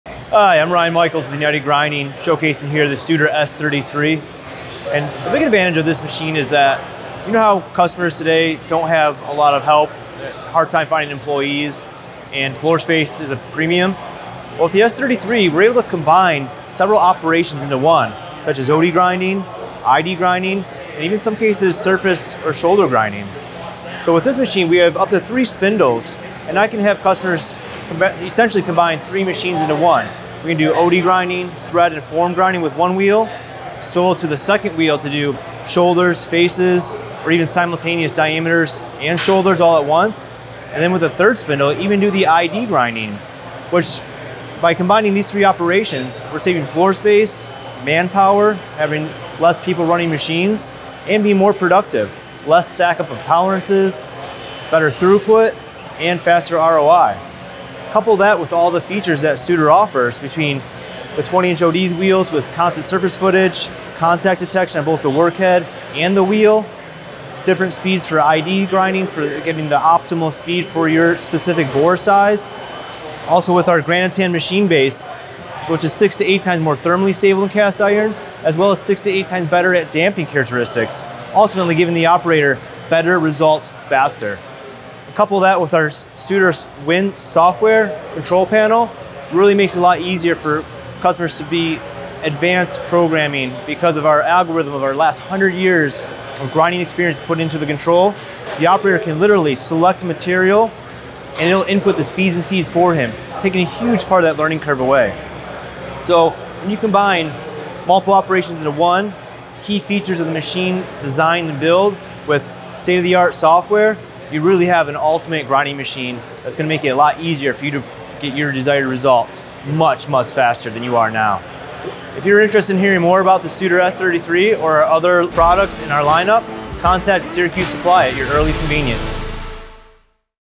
United Grinding Studer S33
Syracuse Supply is your premiere source for CNC machinery, machine tool accessories and supplies.

The S33 is a CNC universal cylindrical grinding machine for the individual and batch production of medium-sized workpieces. Changing over from grinding between centres to live spindle grinding is particularly easy on this machine. The new wheelhead with two motor spindles enables grinding of even more complex workpiece geometries in a single clamping. Its foundation is a machine base made of solid Granitan® S103.

Distance Between Centers: 400 mm / 650 mm / 1,000 mm / 1,600 mm
Center Height:    175 mm
Max Workpiece Weight:  150 kg
Max Travel X:    370 mm
Max Travel Speed X:   15,000 mm/min
Max Travel Z:    500 mm
Max Travel Speed Z:   20,000 mm/min
Max Spindle Power:   8 kW
Max No. of Revolutions:  3,200 /min
Max Grinding Wheel Diameter: 508 mm
Max Grinding Wheel Width: 80 mm